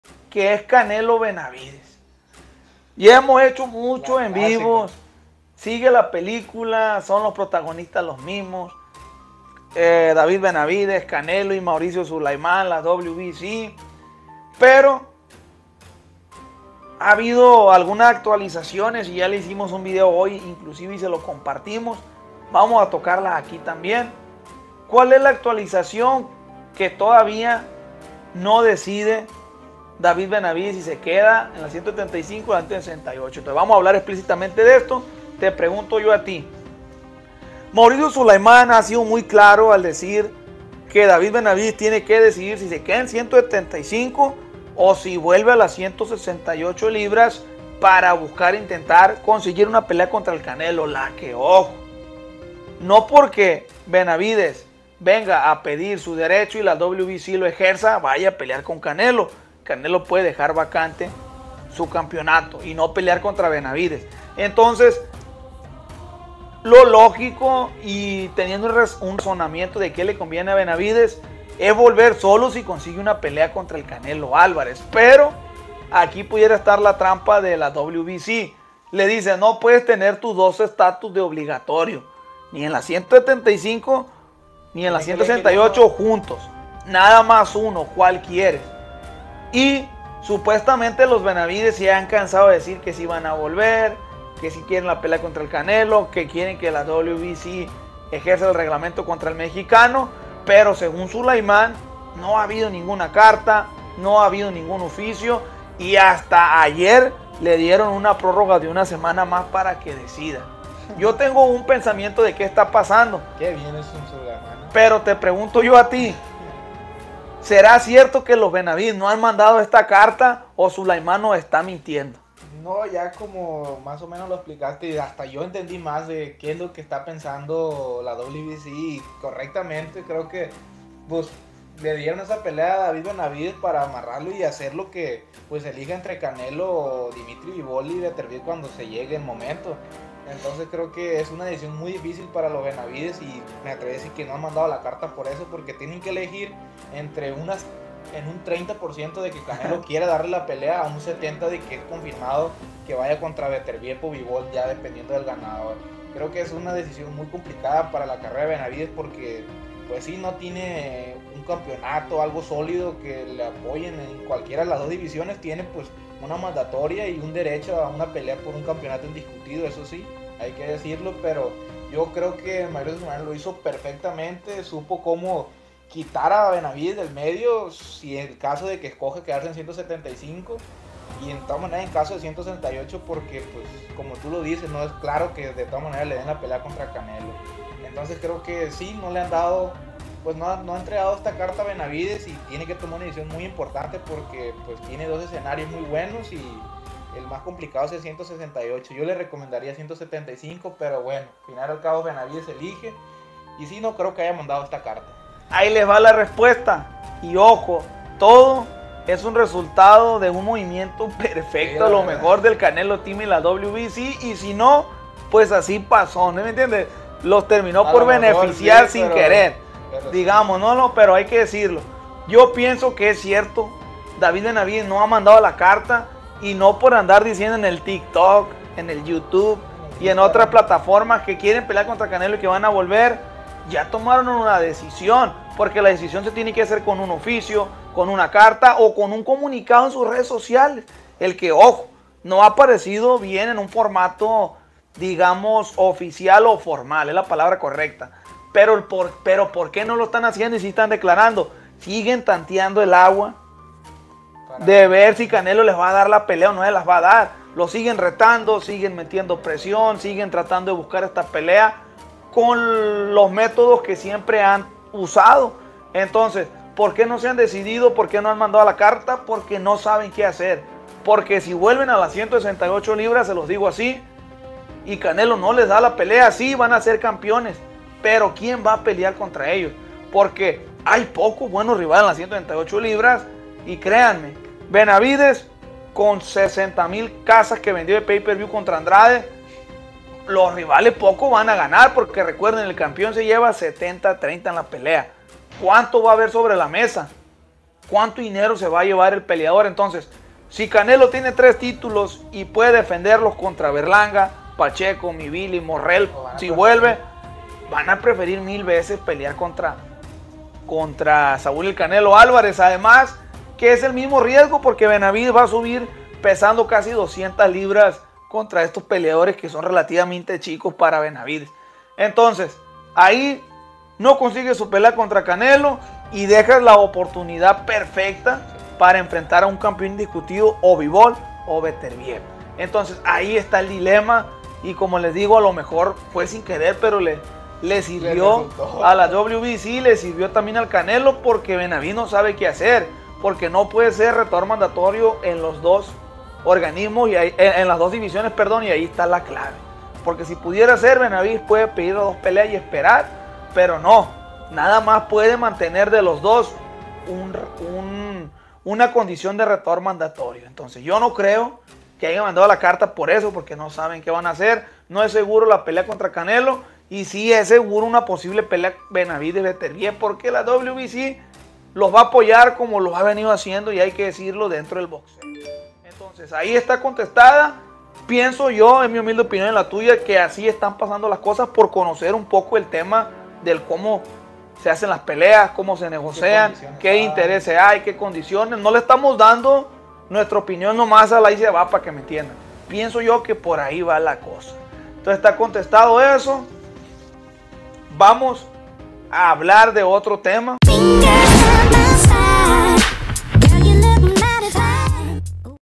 ¡LOS BENAVIDEZ ACORRALADOS! David Benavidez y equipo NO SABEN QUE HACER dentro del boxeo
¿David Benavidez ACORRALADO A PROPÓSITO por Mauricio Sulaiman y el Canelo?‼️